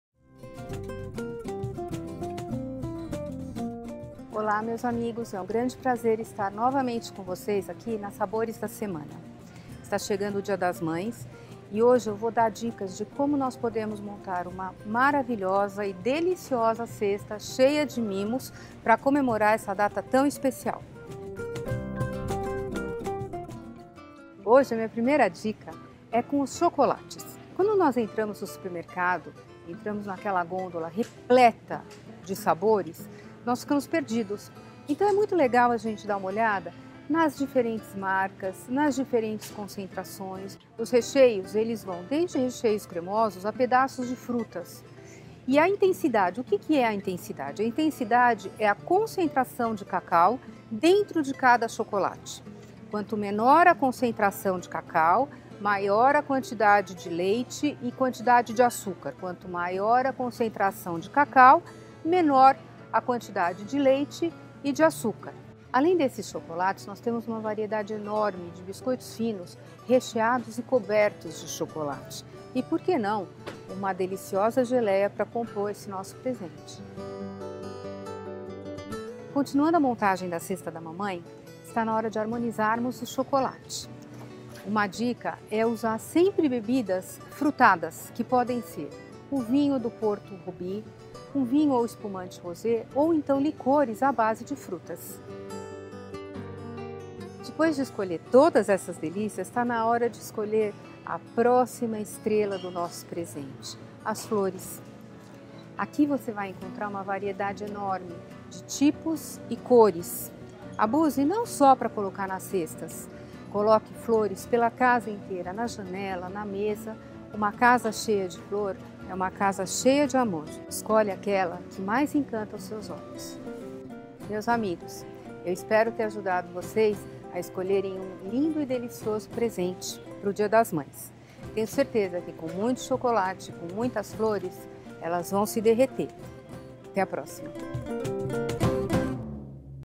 Sabores da Semana :: Quem ama, surpreende!
Já pensou no que você vai dar de presente para sua mãe? No Sabores dessa semana, a chef Carla Elage dá dicas de como surpreender nessa data com flores e chocolates especiais, que podem compor a montagem de uma cesta personalizada e proporcionar uma linda e deliciosa mesa de almoço para as mães.